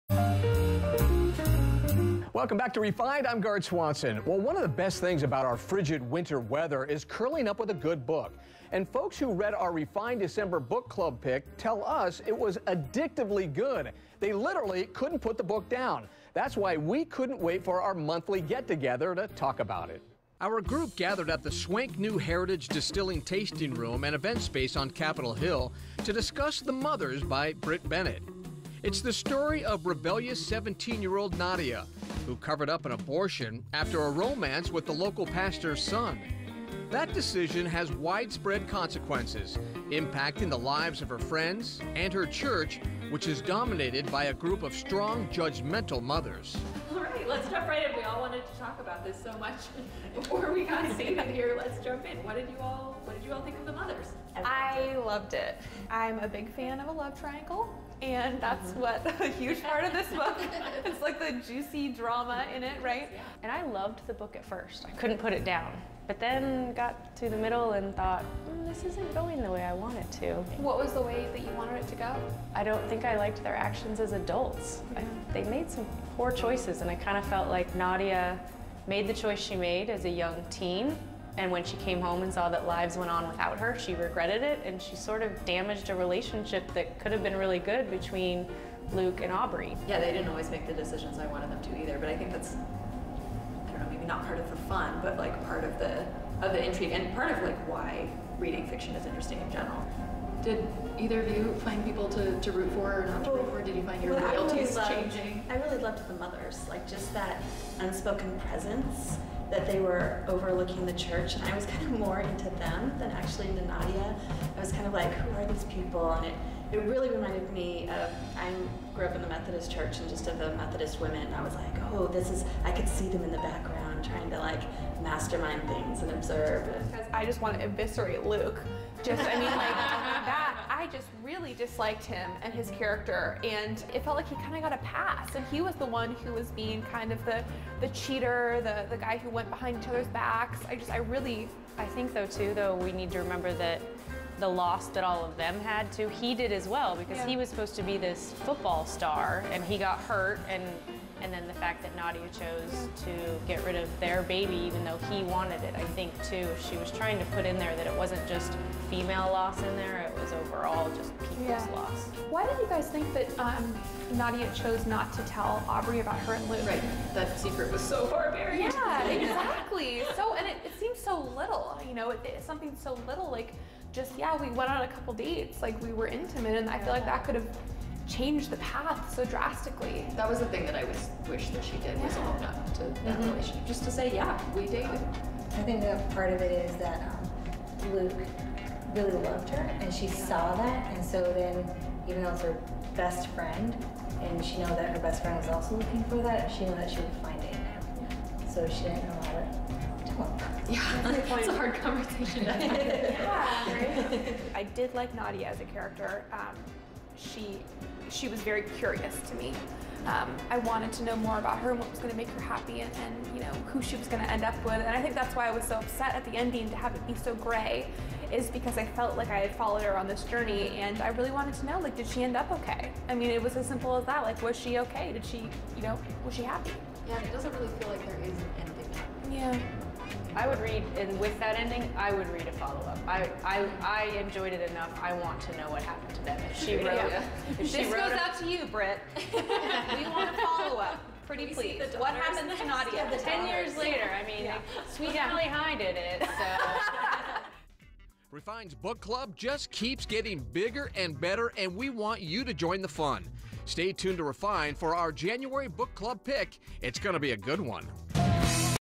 'The Mothers' Discussion Group
We had our discussion group at the Heritage Event Space on Capitol Hill this past week - and it was a doozy!